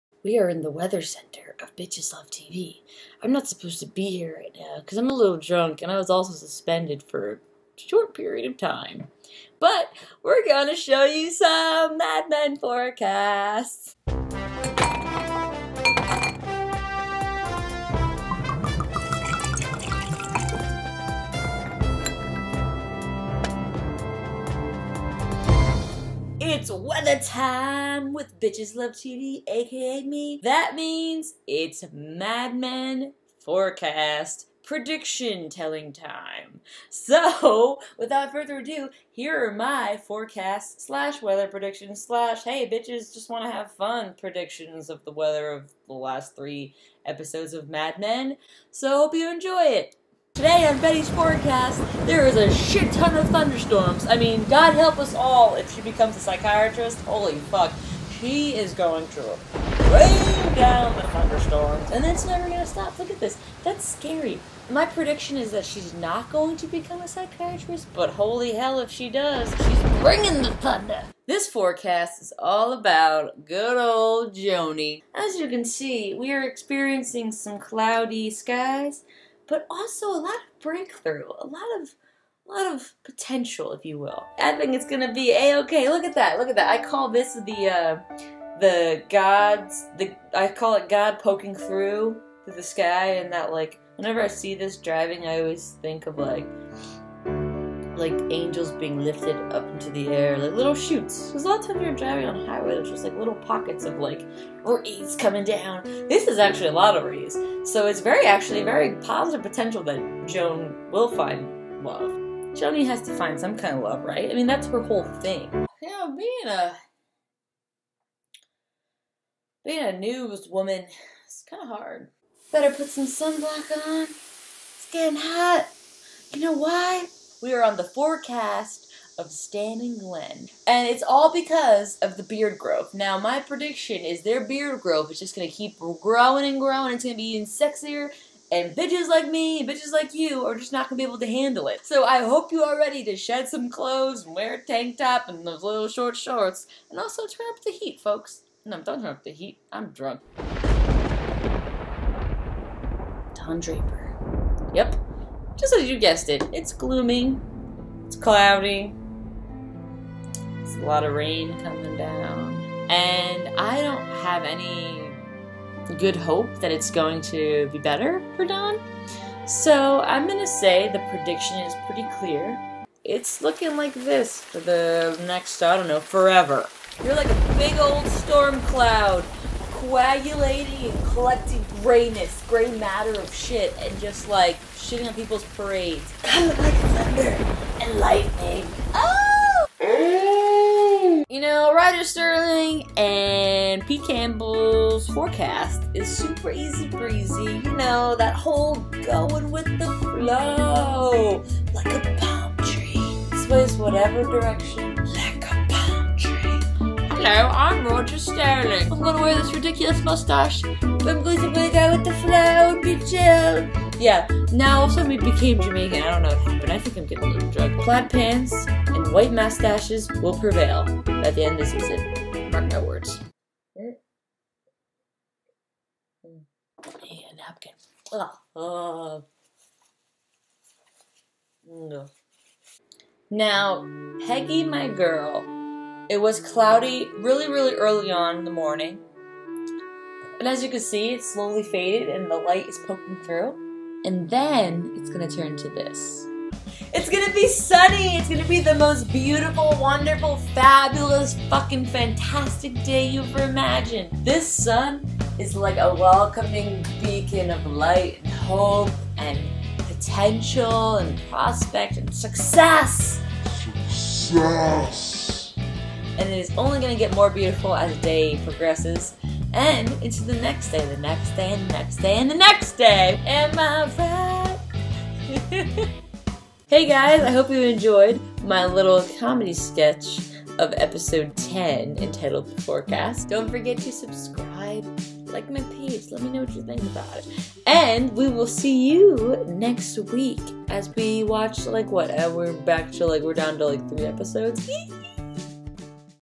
Mad Men S7E10- My Mad Men Forecast!
Mad Men Season 7, Episode 10- The Forecast

I’ve been paying close attention to each episode title in Mad Men season 7, and I must say that this week’s title of the episode is spot on! By titling it “The Forecast,” Matthew Weiner clearly wants you to know that this is a crucial final episode in this riveting television drama, and that this is the time to really give your predictions on what’s going to happen by the end of the series finale. Unlike episode 9 which was quite convoluted and very much lacking in screen time of the best characters (Sally Draper, Pete Campbell and Peggy Olson), episode 10 finally gives us viewers a clearer foreshadowing of what’s going to happen to each character. (And yes, even a pleasant surprise appearance from Glen Bishop!)

So for my video this week, I thought acting as your “Mad Men Weather Woman” would be the perfect way to give you my predictions of what is going to happen by the end of this series finale, while also making it a fun comedy sketch for you to enjoy! My predictions include the following characters on Mad Men: Don Draper, Betty Francis (formerly Betty Draper), Pete Campbell, Joan Harris (formerly Joan Holloway), Roger Sterling, Stan Rizzo, Glen Bishop and last—but certainly not least—Ms. Peggy Olson.  In my “forecast,” I give a foreshadowing of each character by telling you what type of weather they are currently having and will have by the end of the series finale.

Don Draper is the easiest one to predict, (or at least I think so), so let’s use him as an example. Don’s forecast is looking VERY cloudy with heavy rain, scattered thunder storms and a zero percent of sunshine in his future at the ad agency—making his ending on the show overall not hopeful or promising. It looks very unlikely that he will ever manage to get out of the rut he’s in and in my opinion, perhaps that’s just what’s meant to be. (Especially so it will clear an even bigger path for Peggy Olson to take center stage and become the powerful ad-woman she was always destined to be on Mad Men.)

Hope you enjoyed this week’s video and leave me a comment to let me know what you think! Do you agree with my predictions? Do you like how I presented this week’s Mad Men review / comedy sketch?

Directed & Produced by- Vanessa Hargreaves & Bryan Tatninov